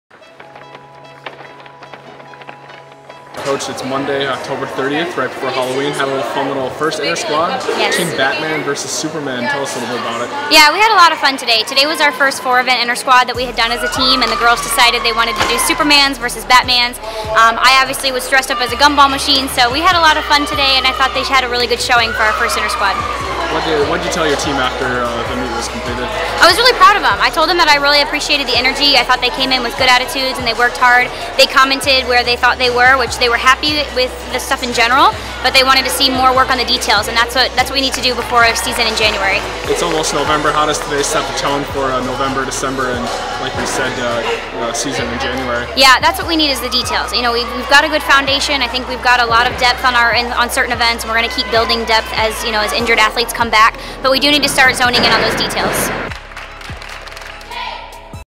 WGYM: Coach Brown - Intersquad Update - 10/30
The Penn State women's gymnastics team held a "Batman vs. Superman" intersquad practice on Monday, Oct. 30.